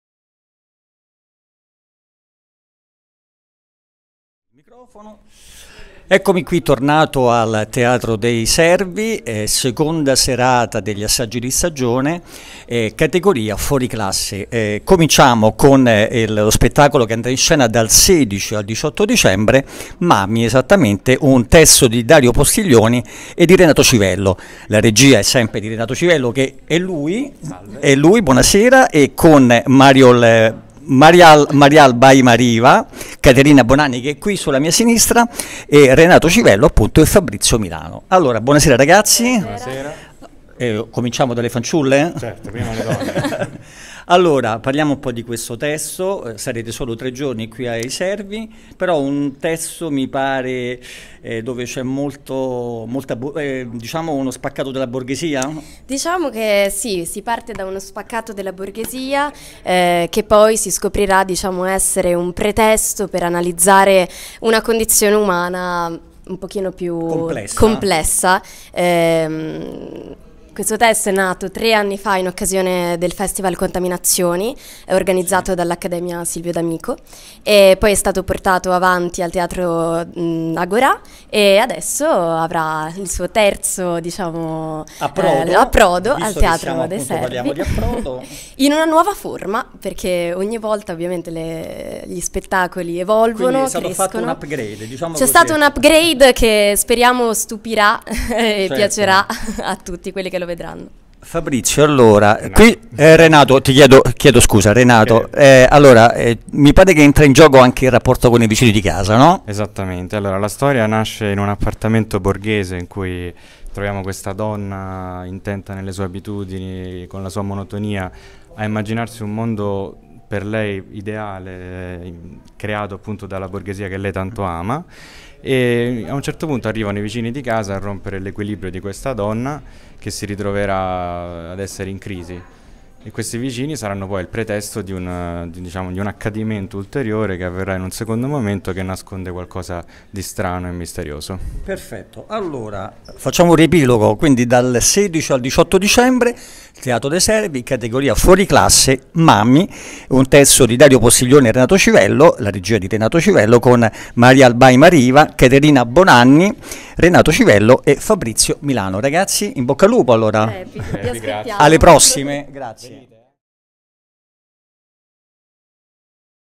Intervista a Renato Civello e Caterina Bonanni
In scena con MUMMY al Teatro De' Servi dal 16 al 18 dicembre 2020